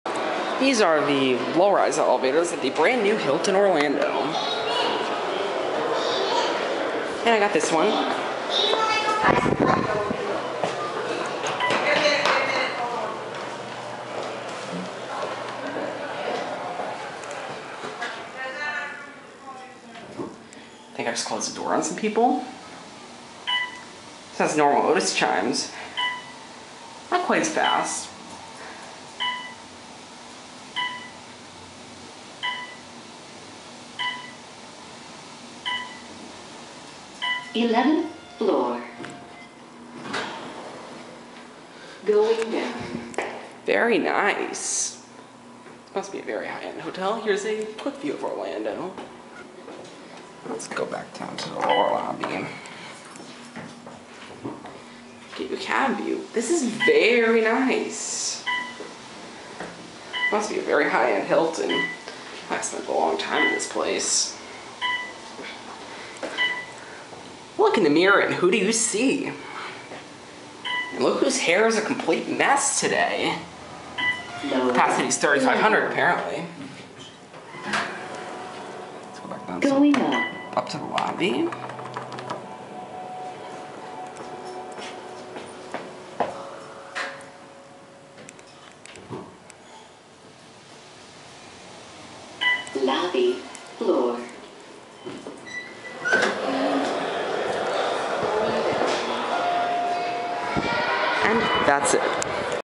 Talking Otis Gen2? Mid Rise Elevators at the Hilton Orlando & Conference Center, Orlando, FL
Installed: 2009
Speed: 400 FPM
Travel Distance: 11 floors
Fixtures: Otis Series 4 Classic